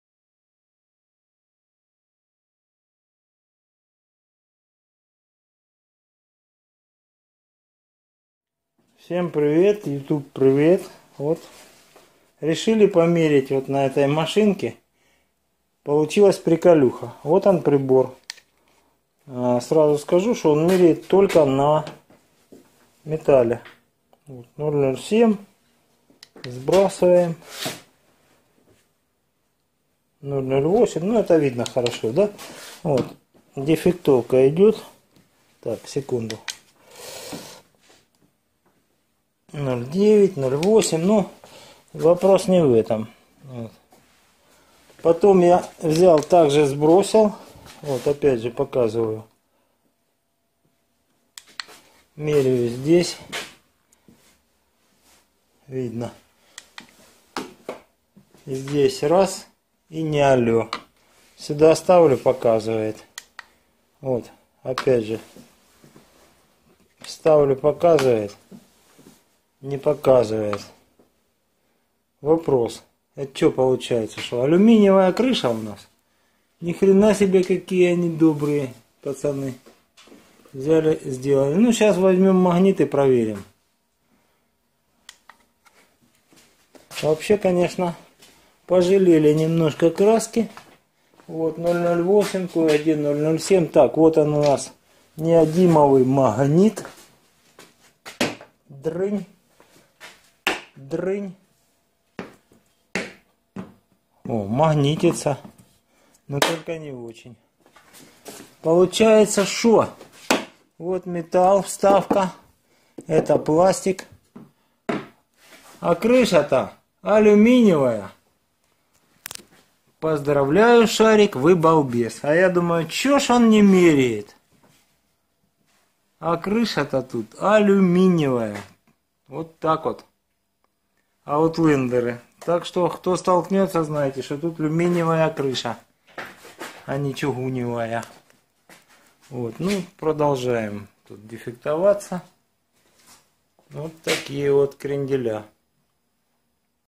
PDR Mitsubishi Outlander Aluminum CEILING - Mitsubishi Outlander алюминиевая крыша
на разных машинах применяют разные кузовные материалы. в данном случае крыша сделана из алюминия. важно когда это обстоятельство выясняется при первичном осмотре а не после когда с клиентом все обговорили и ударили по рукам... потом остается молча делать. будьте внимательны !